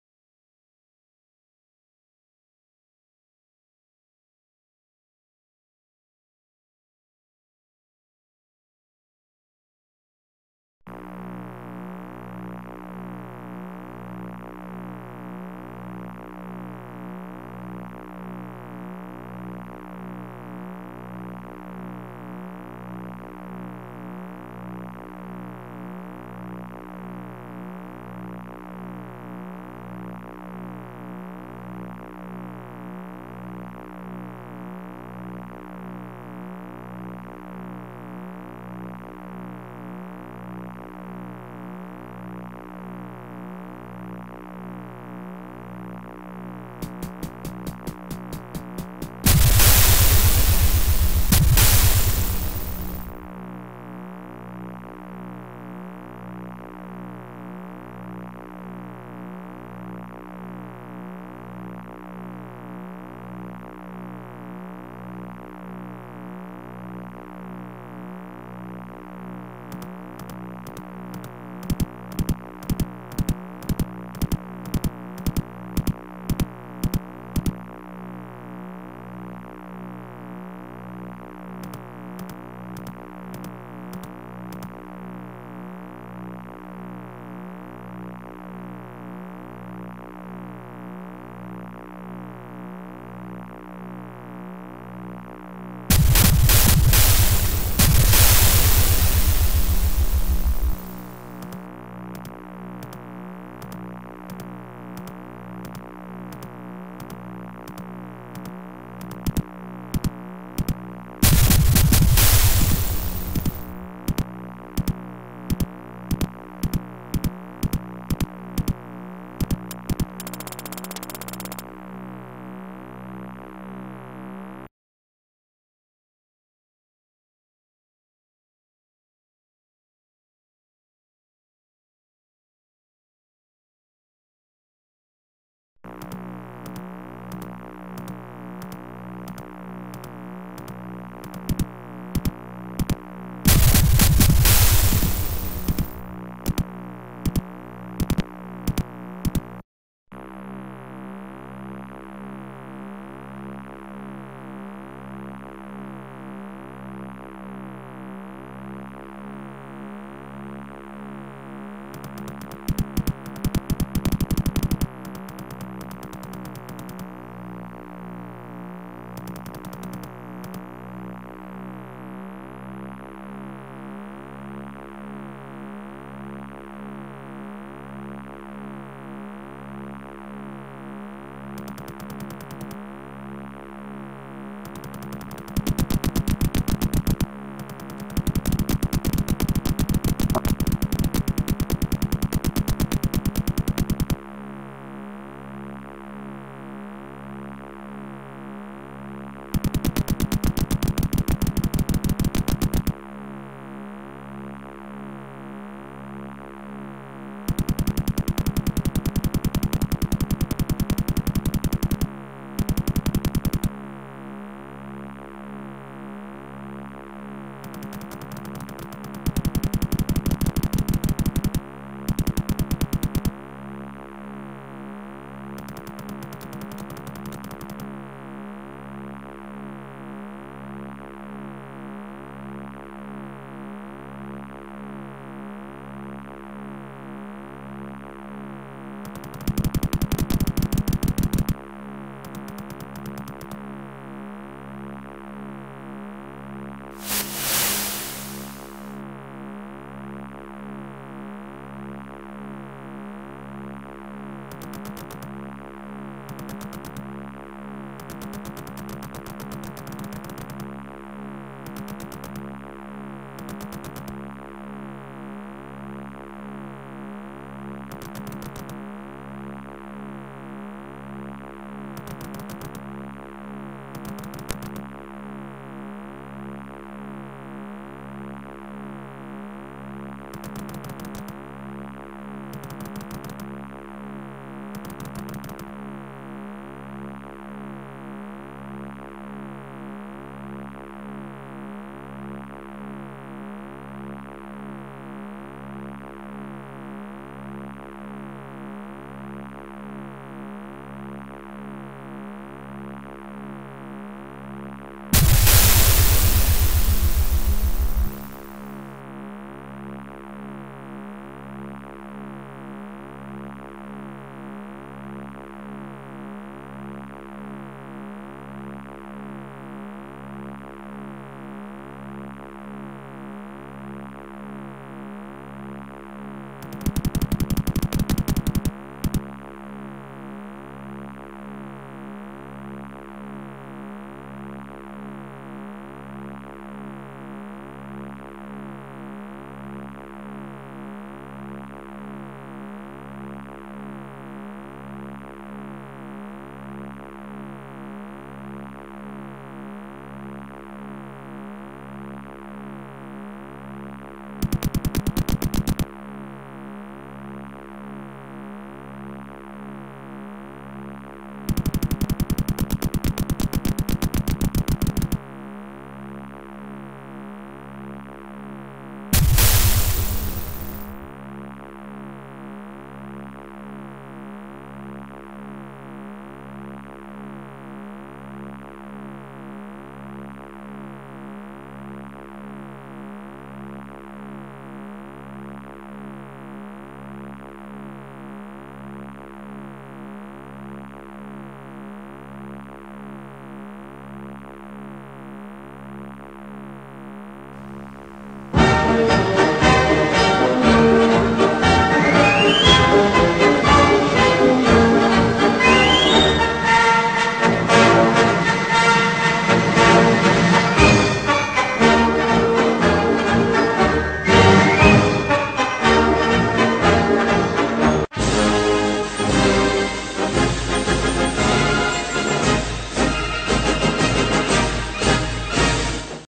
Luftwaffe Bombers. Do 17z-2.
Do 17z-2. All combat flights -1

Combat Flight 1: Bomb the forward airfield of Lympne.